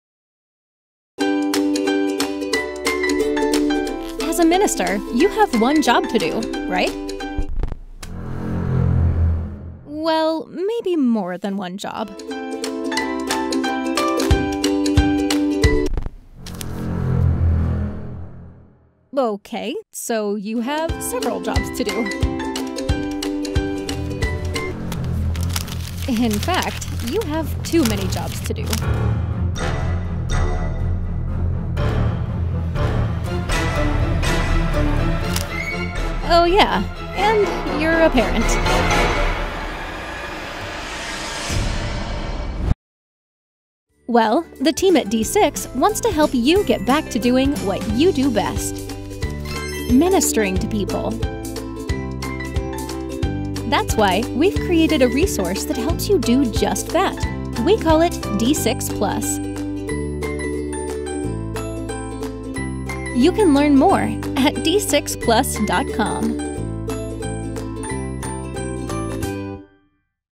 Hello, My Name Is MINISTER
In ministry, do you ever feel like you have TOO MANY jobs to do? D6 now offers a resource to help you take care of those extras, so you can get back to what you do best: ministering to people.